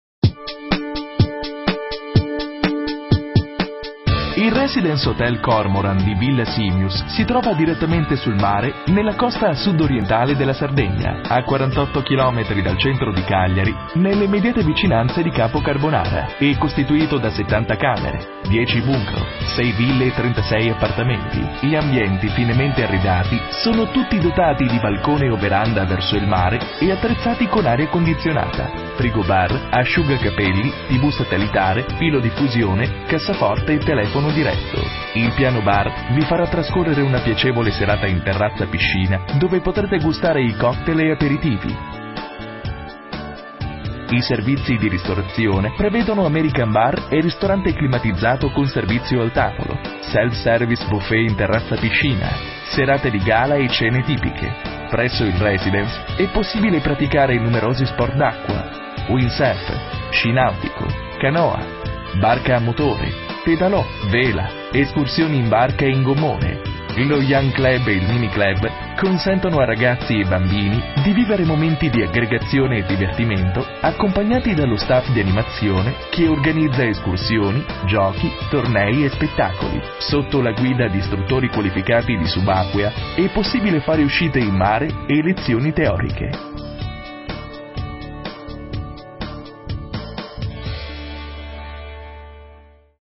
Cormoran
L' albergo Cormoran e' collocato fuori citta',  nella localita' di VILLASIMIUS , in provincia di CA. Dispone di un totale di 63 camere, un numero complessivo di 51 appartamenti.
In tutte le camere sono disponibili svariati servizi: condizionamento dell' aria, le stanze sono dotate di asciugacapelli, per riporre in sicurezza i vostri oggetti di valore avrete a disposizione una cassaforte, un comodo frigobar, presa modem per collegarsi alla rete, ogni stanza dispone di un telefono, la TV per rimanere sempre aggiornati.Nel tempo libero segnaliamo diversi servizi tra cui: per tutta la famiglia, e' disponibile l'animazione, grazie al servizio di biciclette a noleggio potrete spostarvi con facilita', per rilassarvi potrete fare un bagno in piscina, a disposizione di tutti la spiaggia privata riservata ai clienti, per gli sportivi, e' possibile fare sport acquatici, da sottolineare la presenza di campo da tennis.Infine, per gli uomini di affari, sono presenti servizi come:spaziosa sala congressi, moderna sala riunioni.Tra i molti servizi ricordiamo: zone riservate al gioco dei bambini.Si accettano bancomat, carte di credito. Tra le lingue parlate ricordiamo: francese, inglese, spagnolo, tedesco.L' albergo offre tantissimi servizi tra cui: la struttura offre l' aria condizionata in tutti gli spazi comuni, per un drink in qualsiasi ora della giornata si puo' usare il bar dell'hotel, per gli appassionati di shopping sono presenti una boutique/negozi, per parcheggiare la vostra auto, la struttura dispone di un parcheggio esterno, per i momenti di relax, e' presente un parco/giardino, un servizio di baby sitting rendera' piu' piacevole il vostro soggiorno, tra i servizi ricordiamo la colazione a buffet , Per i buongustai un ristorante interno di ottima qualita'.